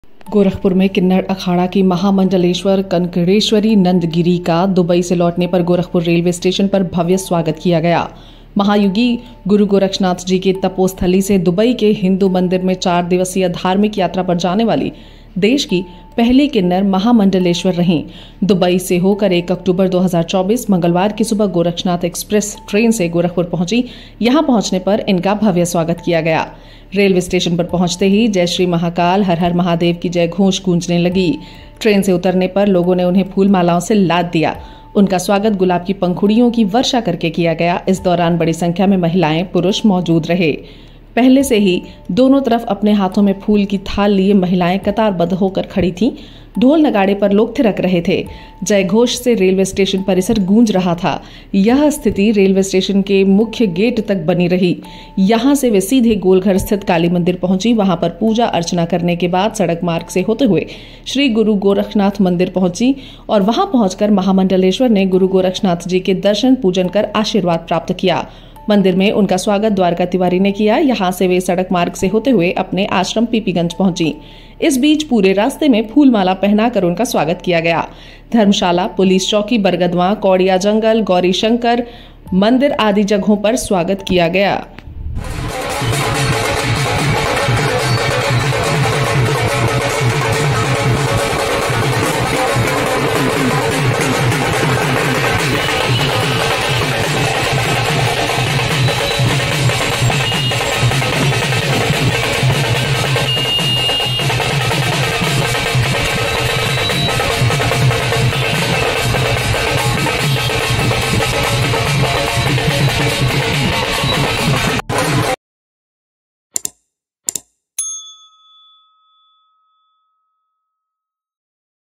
गोरखपुर | किन्नर अखाड़ा की महामंडलेश्वर कनकेश्वरी नंद गिरी का भव्य स्वागत, दुबई यात्रा के बाद लौटीं
दुबई से लौटने के बाद गोरखपुर रेलवे स्टेशन पर किन्नर अखाड़ा की महामंडलेश्वर कनकेश्वरी नंद गिरी का भव्य स्वागत किया गया। महायोगी गुरु गोरक्षनाथ जी की तपोस्थली से जुड़ीं, कनकेश्वरी नंद गिरी देश की पहली किन्नर महामंडलेश्वर हैं, जो दुबई के हिंदू मंदिर में चार दिवसीय धार्मिक यात्रा पर गई थीं। गोरखनाथ एक्सप्रेस ट्रेन से गोरखपुर पहुंचने पर जय श्री महाकाल और हर हर महादेव के जयघोष के साथ गुलाब की पंखुड़ियों की वर्षा कर उनका स्वागत किया गया। इसके बाद वे गोलघर स्थित काली मंदिर और श्रीगोरखनाथ मंदिर गईं, जहां उन्होंने पूजा-अर्चना की और आशीर्वाद प्राप्त किया।